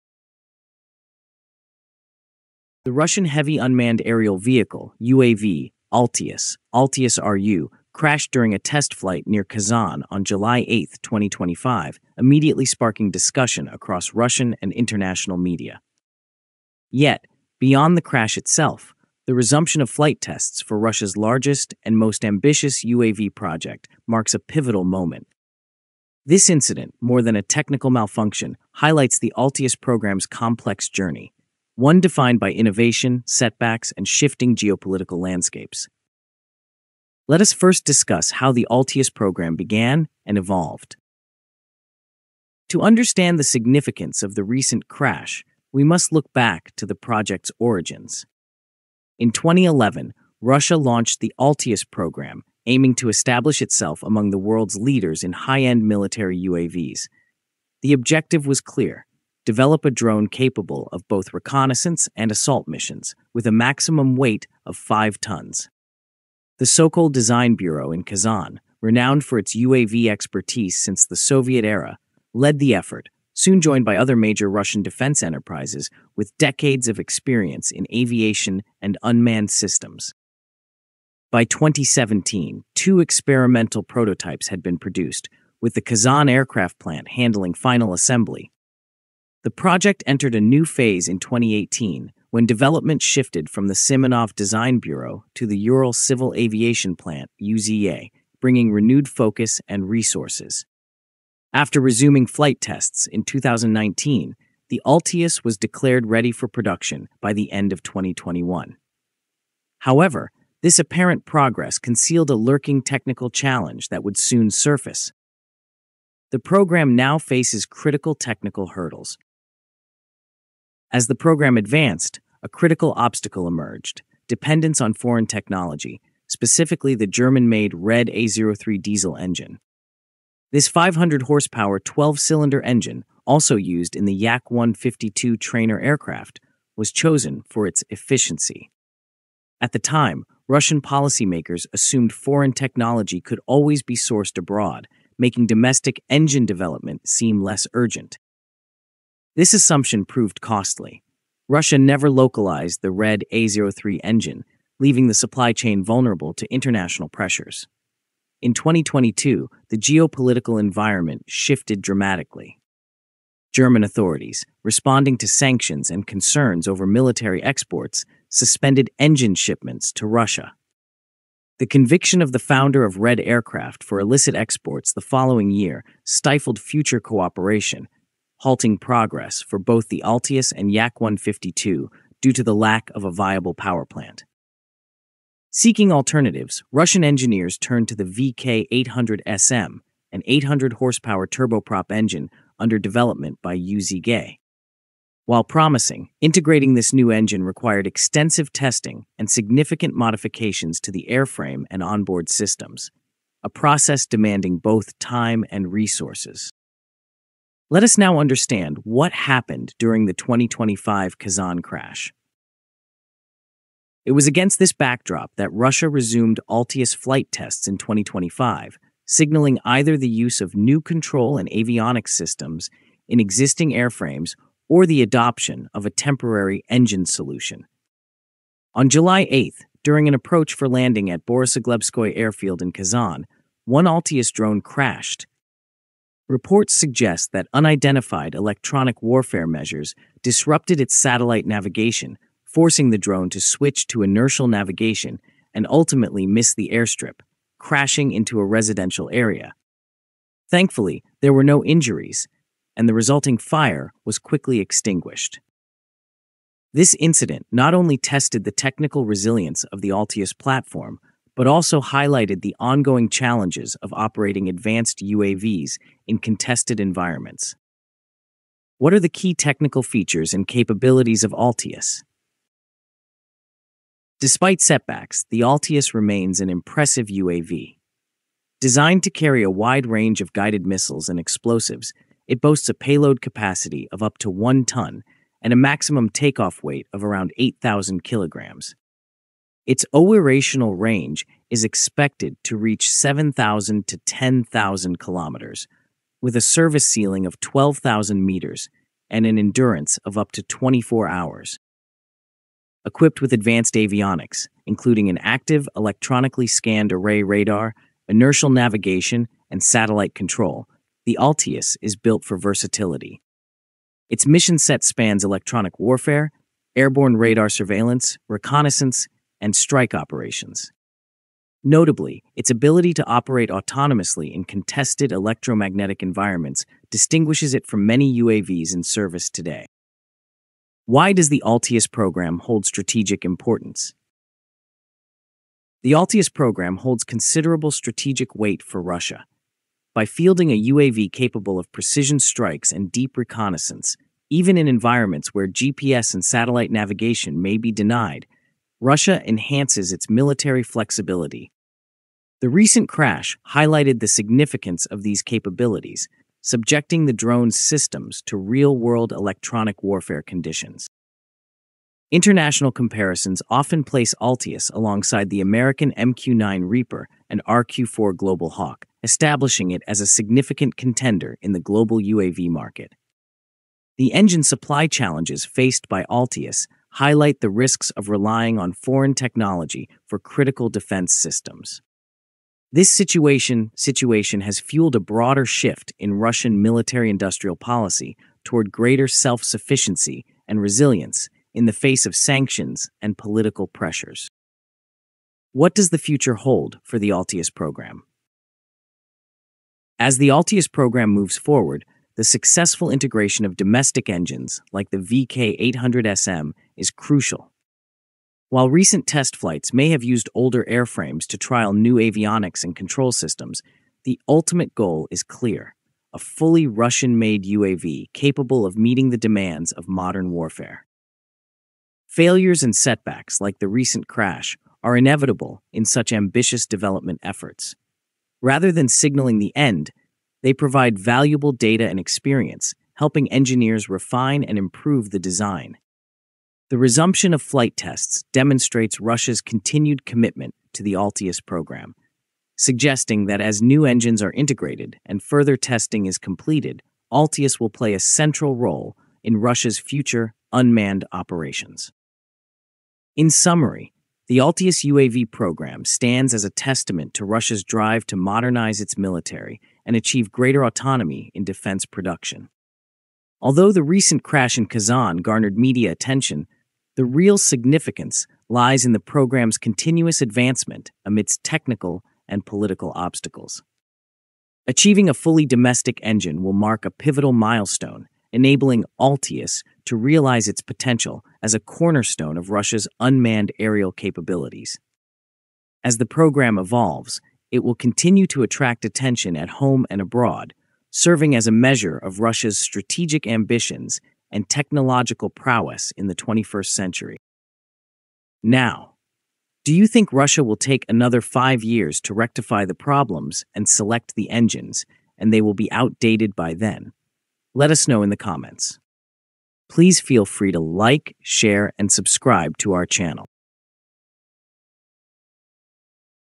VK-800SM: Can This Engine Save Russia’s Future Recon-Strike Drone?
On July 8, 2025, Russia’s advanced heavy UAV "Altius" underwent a critical test flight near Kazan. While the mission encountered challenges, the program continues to push forward, showcasing Russia’s commitment to cutting-edge unmanned technology.

🔹 In This Video:
✔ The Evolution of Altius – From early prototypes to modern avionics and engine integration
✔ Strategic Importance – Why this UAV is a game-changer for Russia’s Aerospace Forces and Navy
✔ Altius vs. Global Competitors – How it compares to the MQ-9 Reaper and RQ-4 Global Hawk
✔ Overcoming Sanctions – Russia’s shift to domestic engines (VK-800SM) and self-reliance
✔ Future Missions – Reconnaissance, strike capabilities, and electronic warfare roles

The Altius program represents Russia’s push for technological sovereignty in high-end military UAVs. Despite obstacles, this project proves that innovation and adaptation are driving Russia’s defense industry forward.